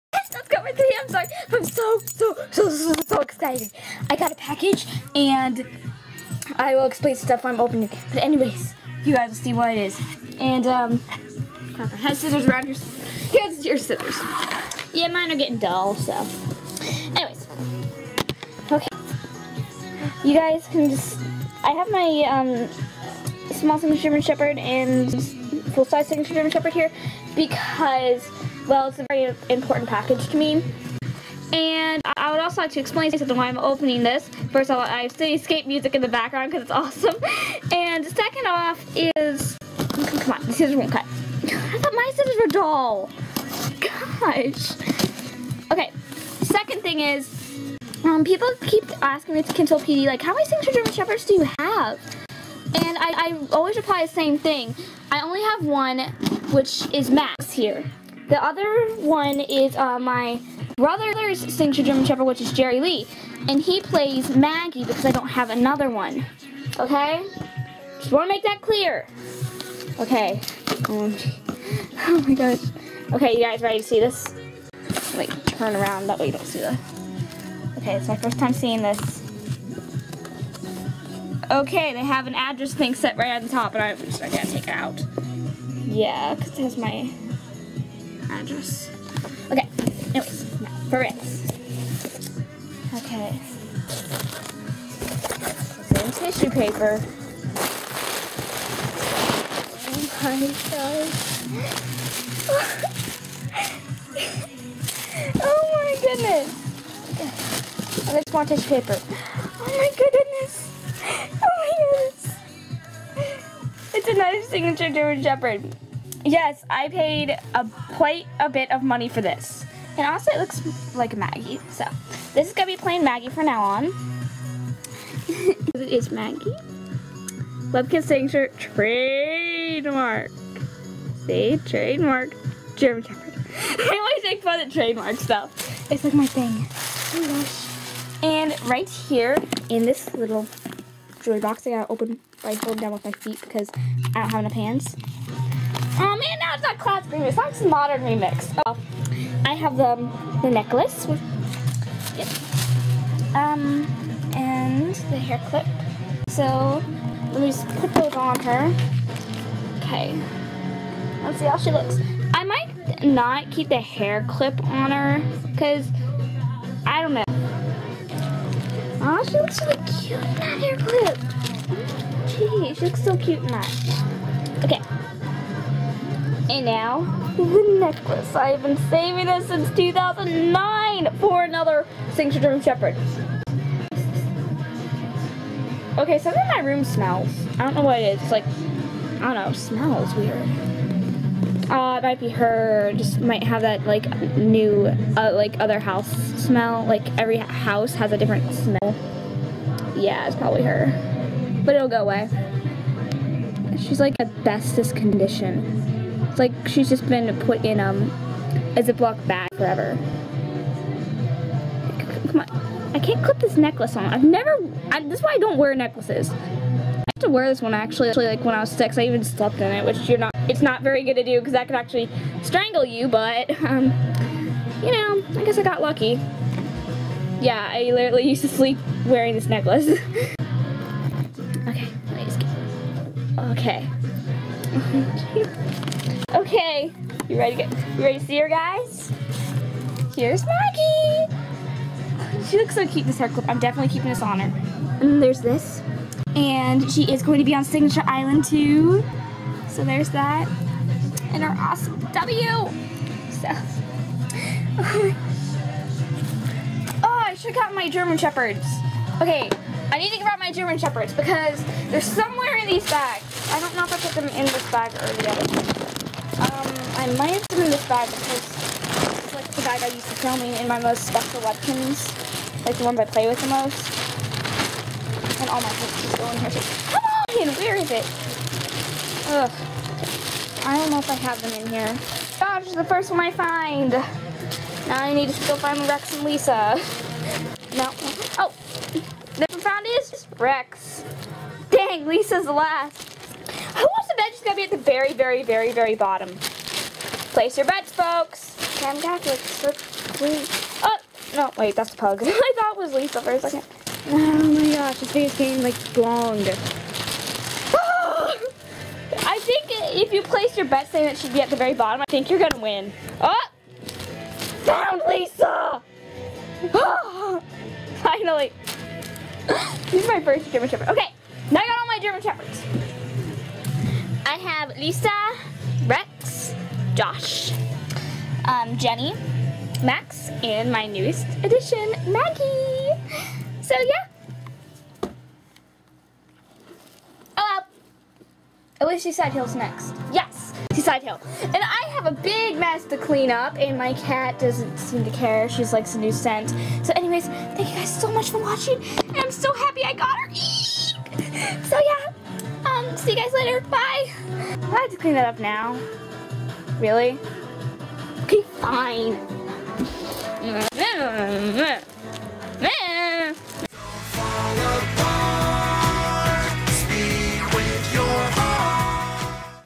Opening My New Webkinz Signature German Shepherd
OMG I love Maggie so much!!! :DDDDD
Oh, and I was gone all day, so I couldn't upload this sooner. :/ Sorry!